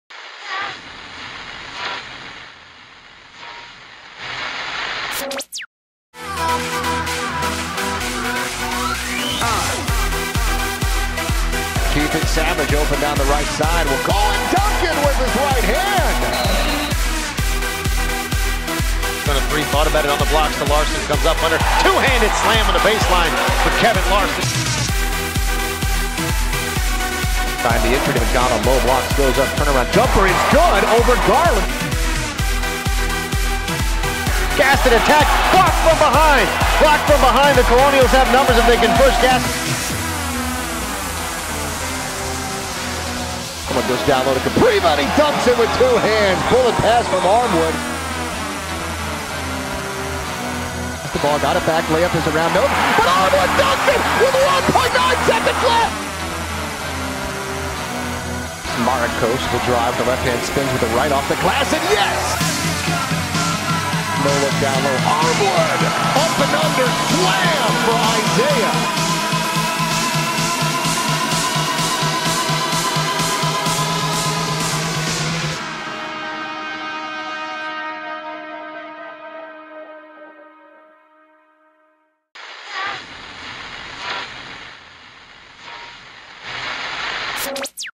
GW Men's Basketball: A-10 Home Schedule Release
NOTE: GW vs. Mason on 1/25 has been rescheduled for a 12pm start time on NBCSN. Check out this video to get the rundown of home A-10 games at the Smith Center this year. VCU, Mason, La Salle and many others are slated for a trip to Foggy Bottom.